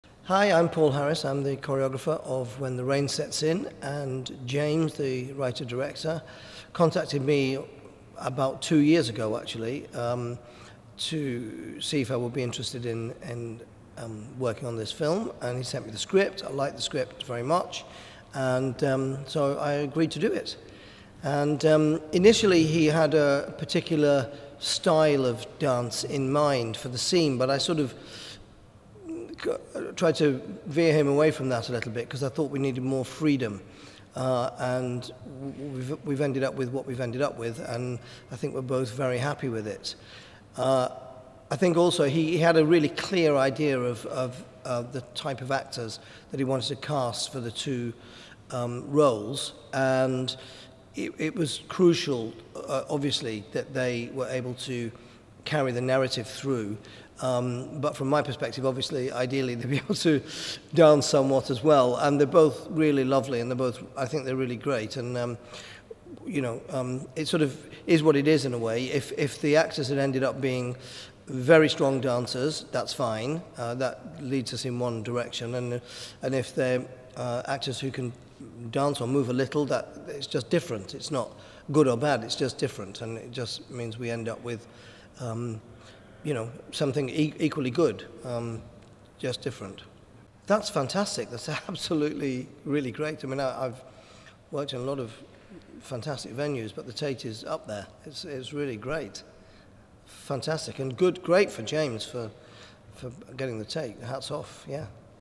When the Rain Sets In - An Interview with our Choreographer
WHEN THE RAIN SETS IN | Kenzie and Treyden, a couple who’ve been swept up in a whirlwind six-month romance, find themselves confronting a painful question: Has their love story come to an end?

During our Tate Britain shoot, our BTS team took time out to record an interview with our Choreographer Paul Harris.
Paul discusses the key decisions and approaches taken with our moonlight dance sequence.

CAST
Treyden    TOBY SEBASTIAN
Kenzie      FLORENCE HOWARD

Writer Director   JAMES HUGHES
Cinematographer   ANDREAS NEO
Choreographer   PAUL HARRIS
Editor   PATRICK J DON VITO
Composer   JEAN-PASCAL BEINTUS
Production Company   SUNSET APERTURE